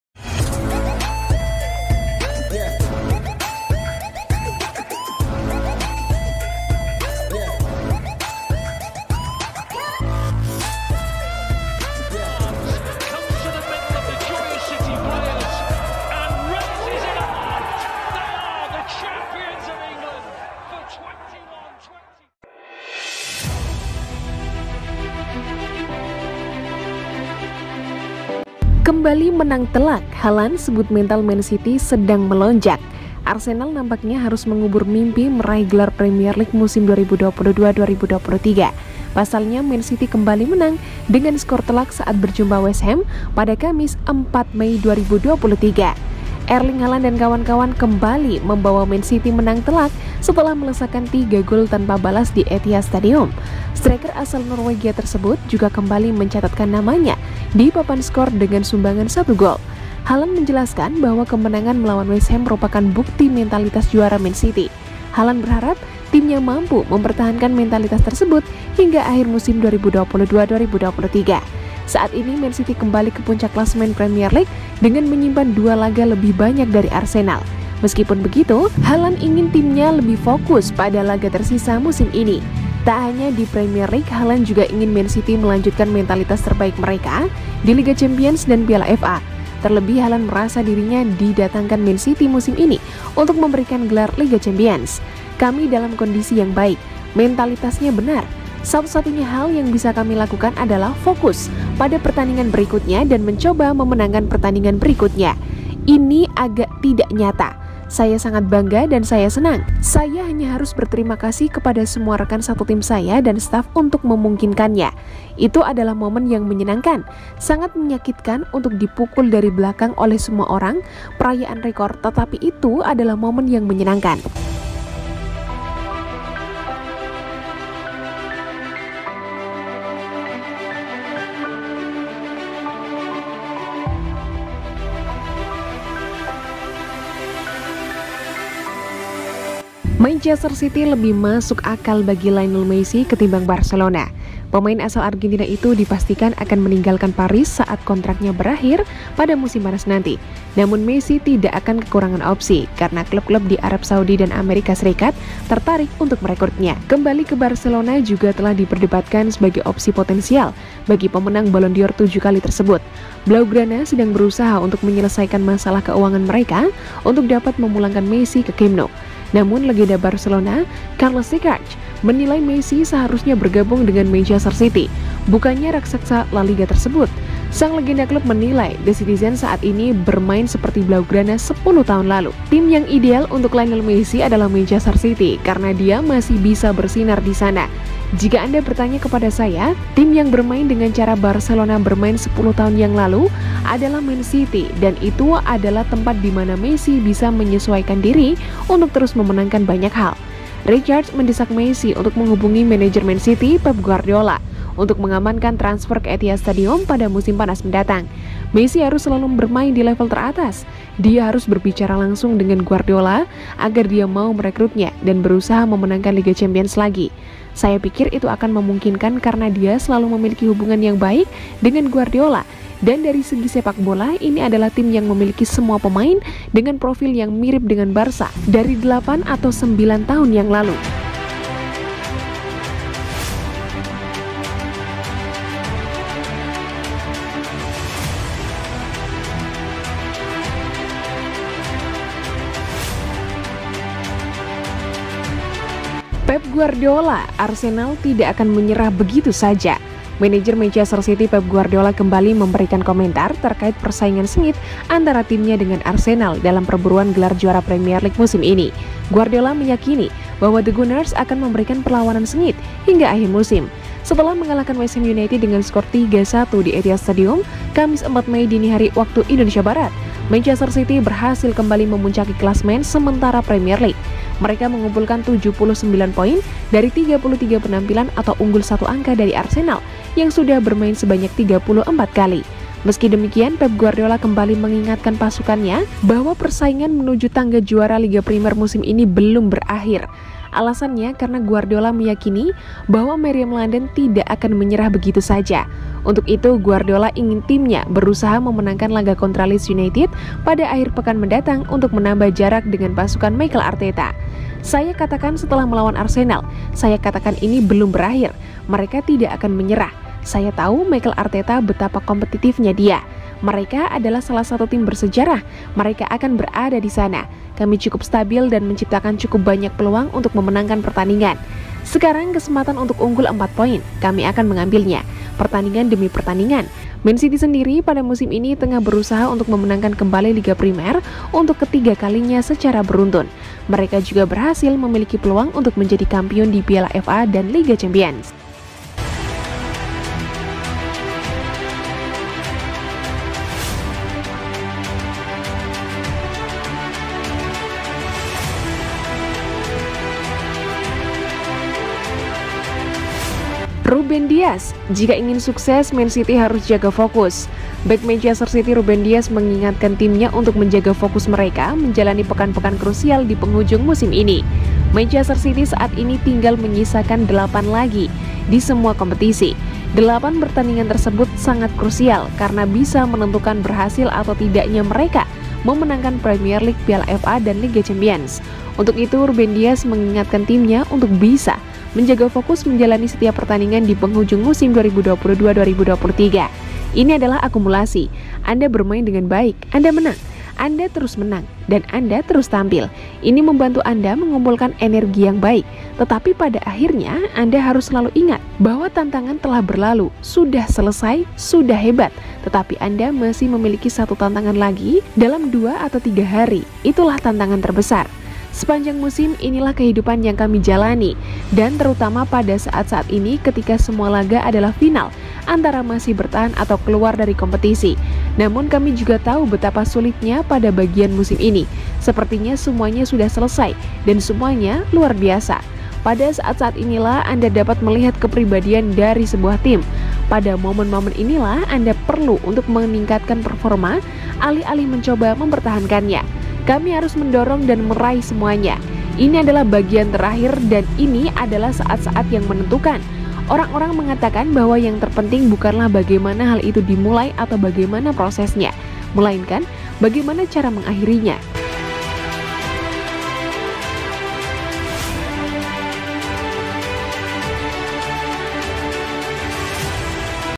DATANGKAN MESSI !! Messi Lebih Pantas Ke Mancity📝Haaland : City Sedang On Fire🔥Wajib Fokus Juara🏆
INFO BERITA : DATANGKAN MESSI !! Messi Lebih Pantas Ke Mancity📝Haaland : City Sedang On Fire🔥Wajib Fokus Juara🏆